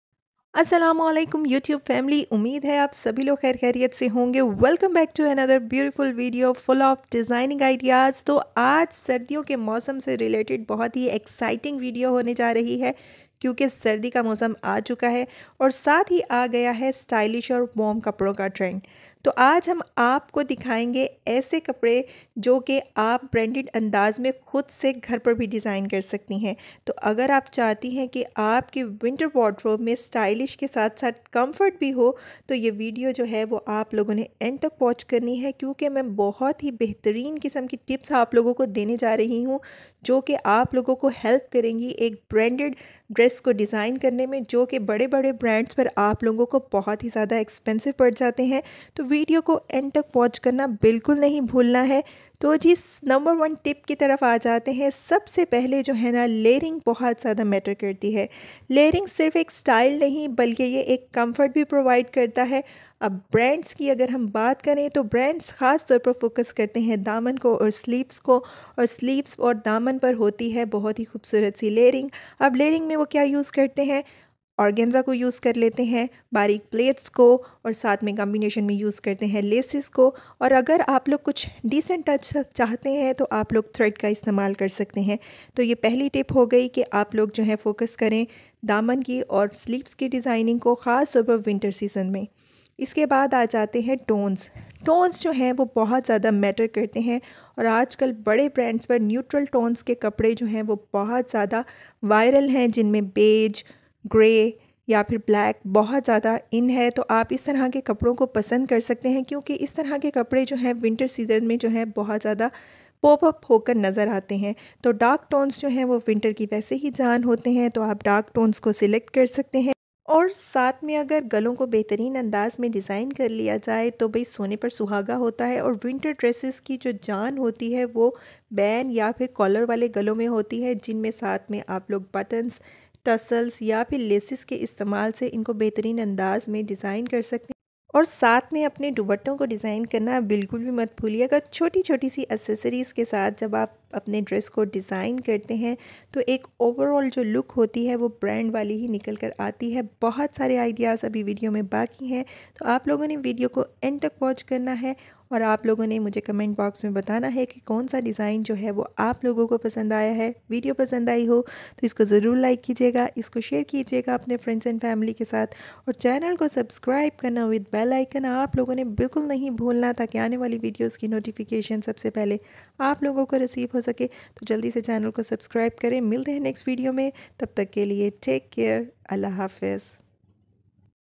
Sardiyon K Kapron Ko Design Karien Branded Andaaz Mei || Design winter Dresses In Branded Style
in this video i am sharing with you how you can design your winter dresses in branded styles.you will get many ideas of different brands how they have designed their winter collection..how they have designed neck,daman and sleeves..you wil see many diffrent styles of daman design sleeve design and neck designs winter dress design